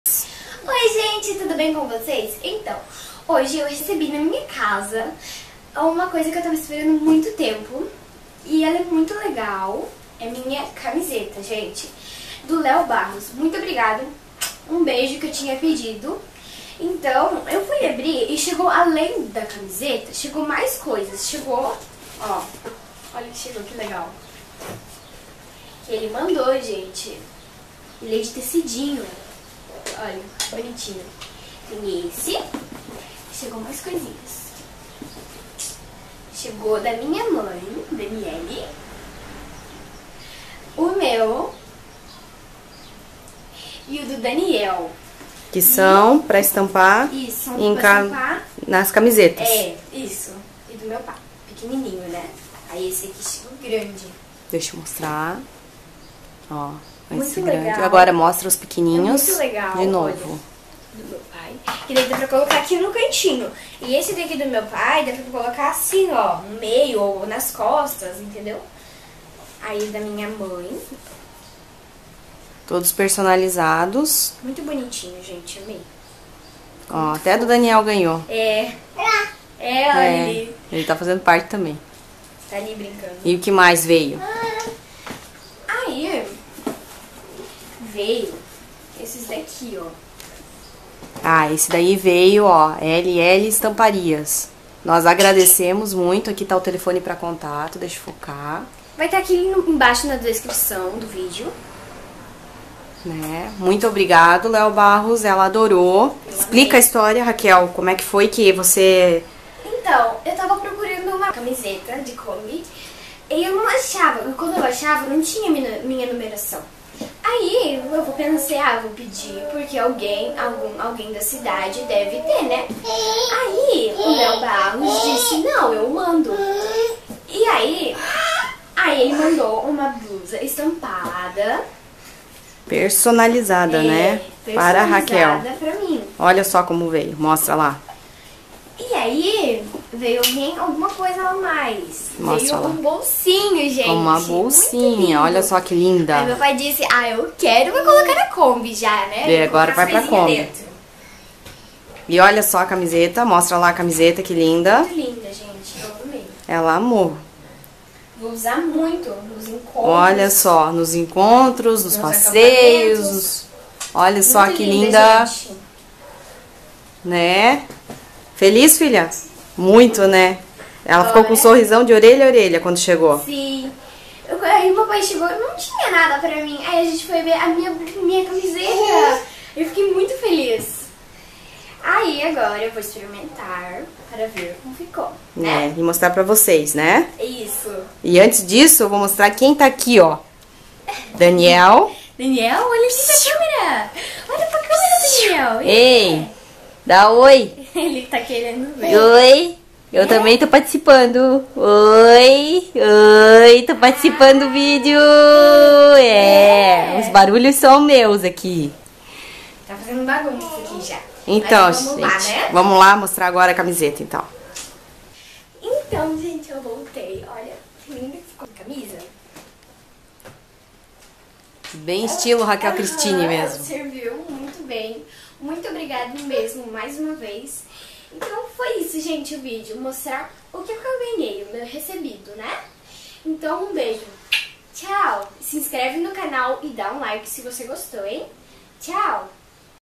Raquel Recebeu Presente de Seguidor - Kombi Cristini
Raquel ganhou uma camisa personalizada de um seguidor.